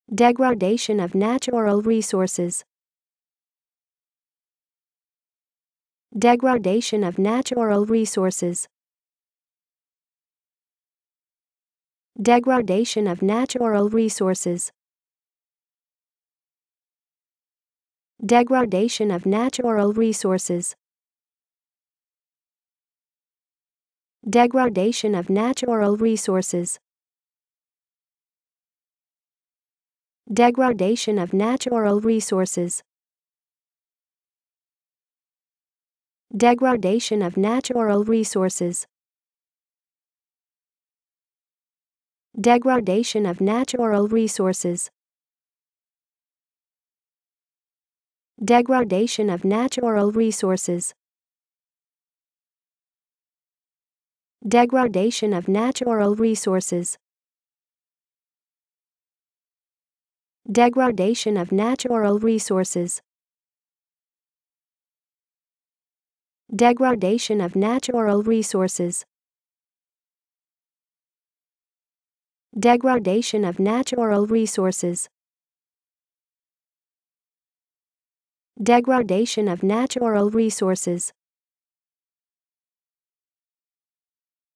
Como dizer 'degradação dos recursos naturais' em ingles?
Como dizer degradação dos recursos naturais em Ingles? Como se pronuncia degradation of natural resources? How to pronounce degradation of natural resources?